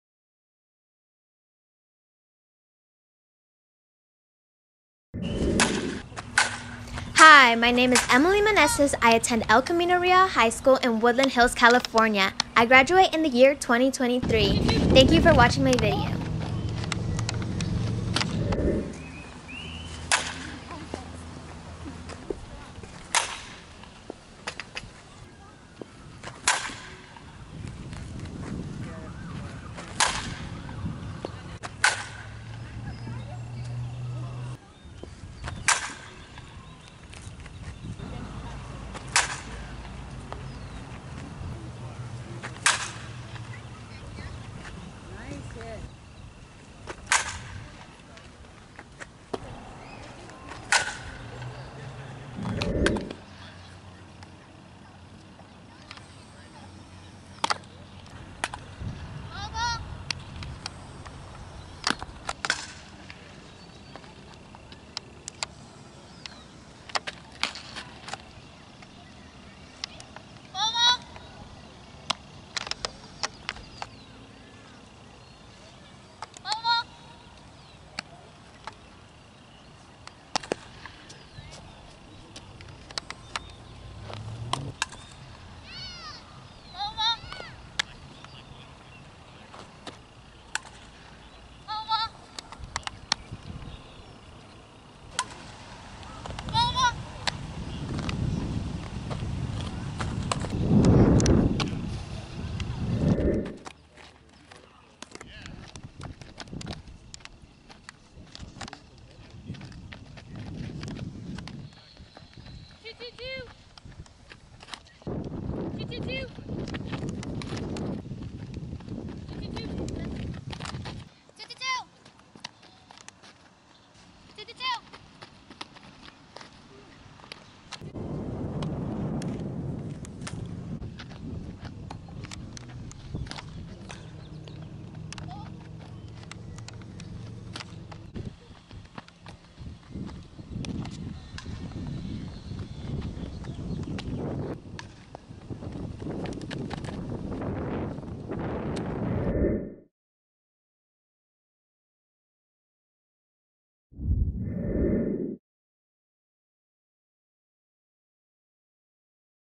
2023 Emily Meneses Outfield and 2B Softball Skills Video - So Cal Choppers - Rohr
Emily Meneses
2023
OF/2B  Bats/R Throws/R
El Camino Real Charter High School, Woodland Hills, CA
Travel Team: So Cal Choppers - Rohr
GPA: 3.5